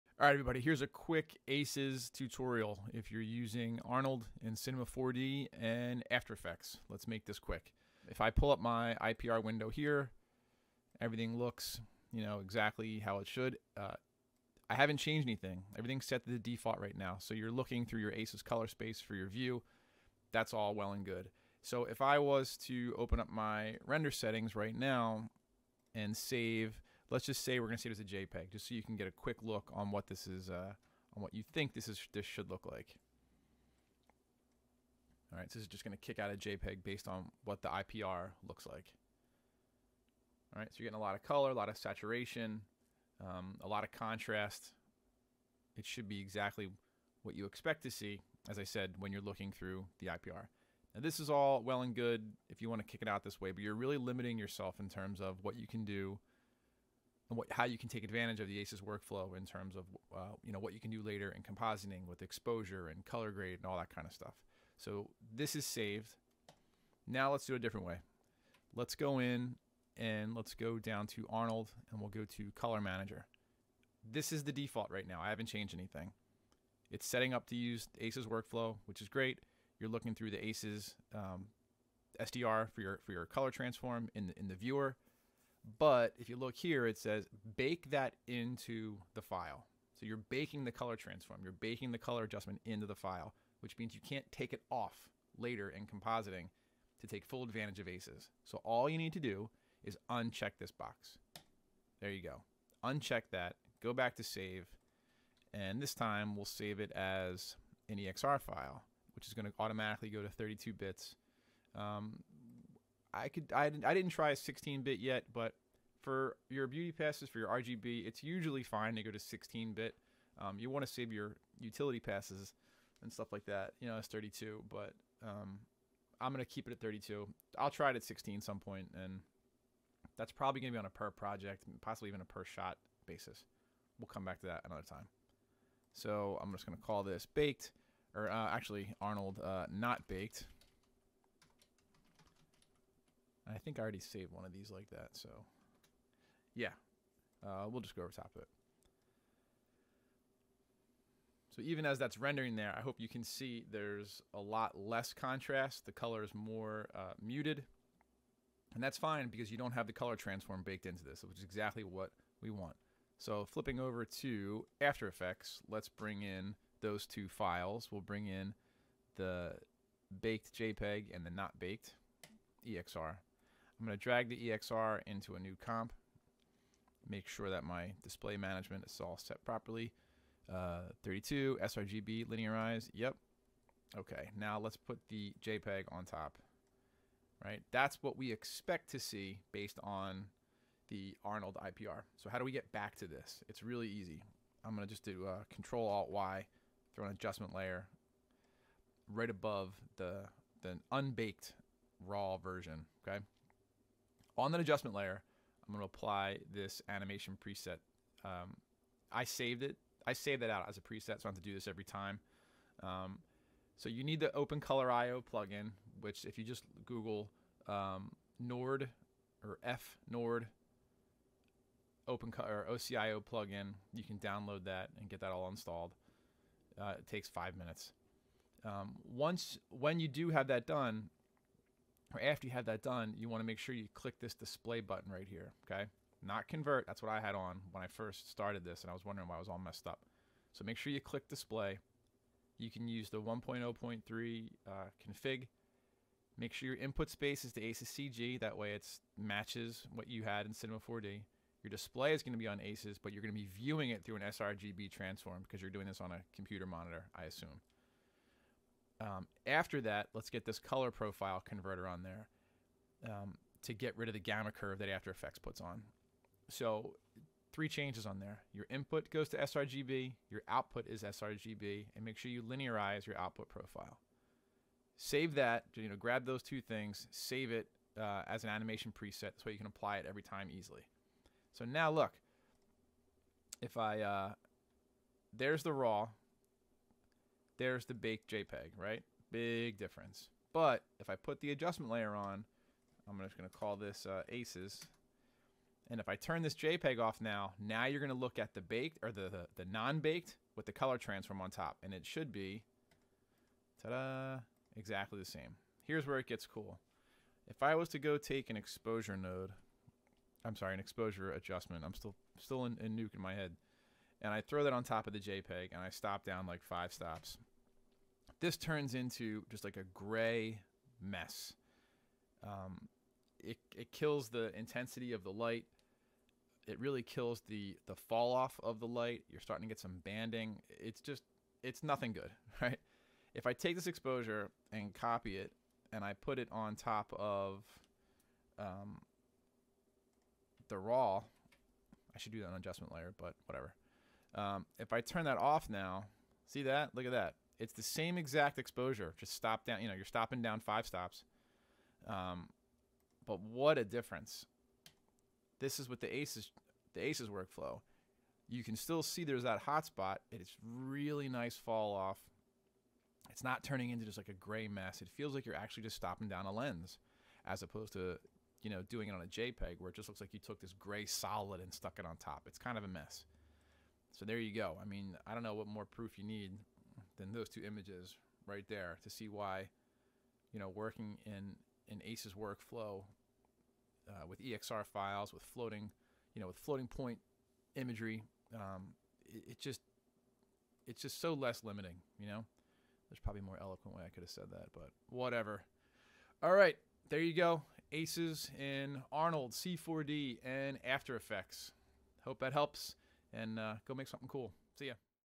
ACES for Cinema 4D, Arnold, and After Effects in 5ish minutes.
In this video, we will look at three short steps to setup a proper ACES workflow using Cinema 4D, Arnold, and After Effects.